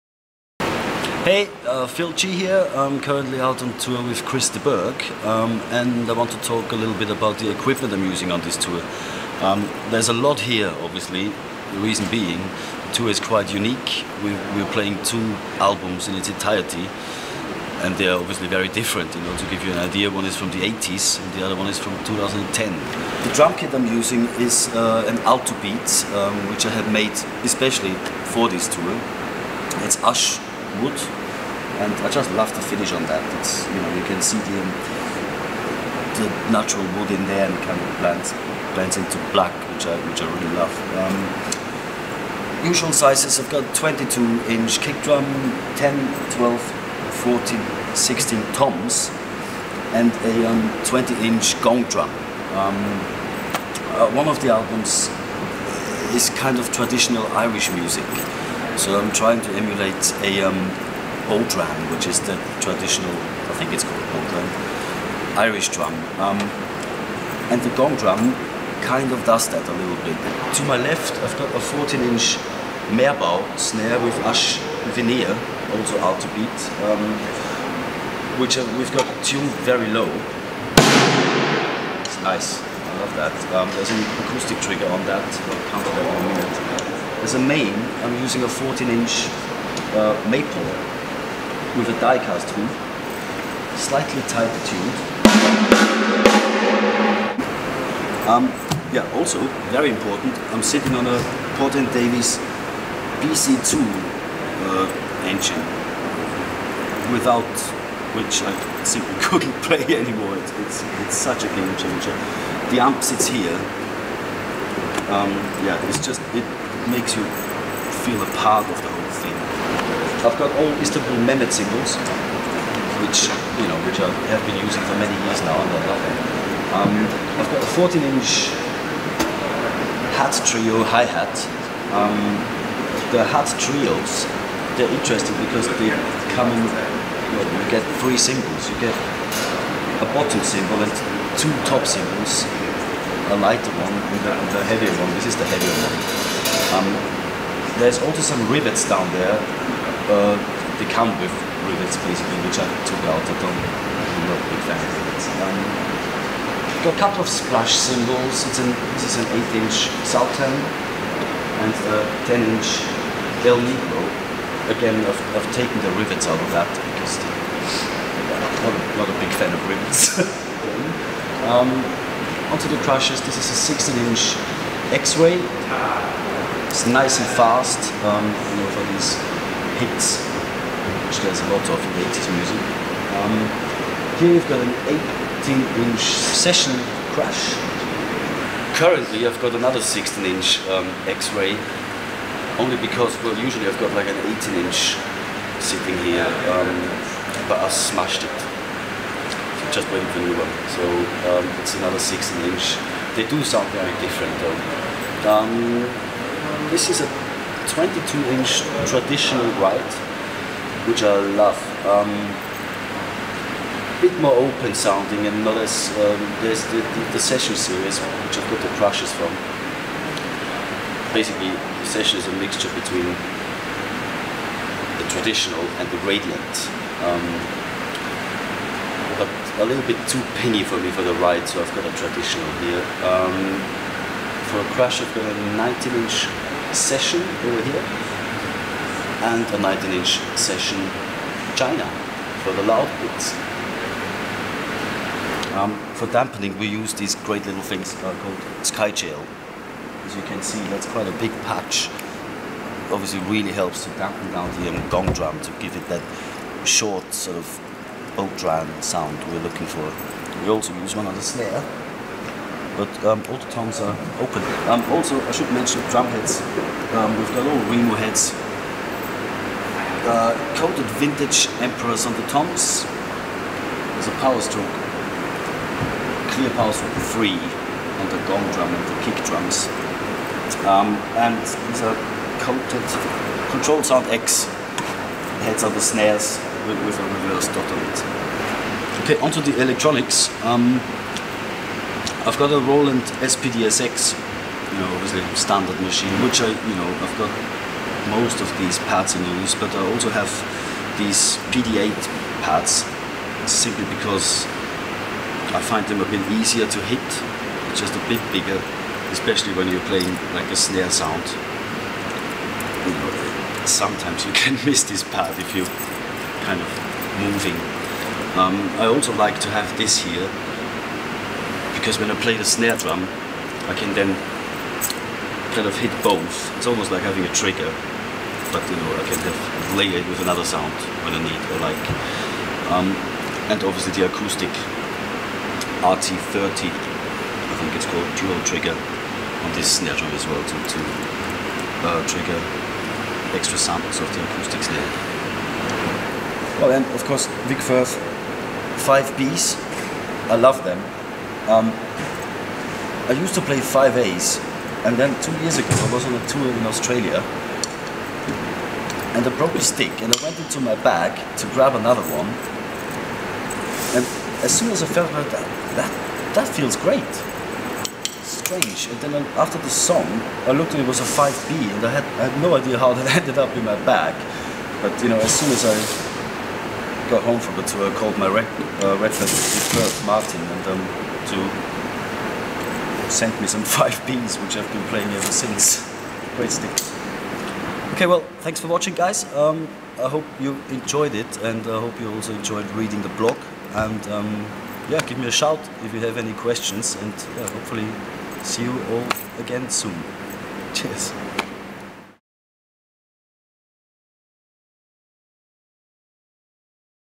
Philipp Groyssboeck - Set up walkthrough
Istanbul Mehmet Artist Philipp Groyssboeck takes us through his 2019 set up with Chris De Burgh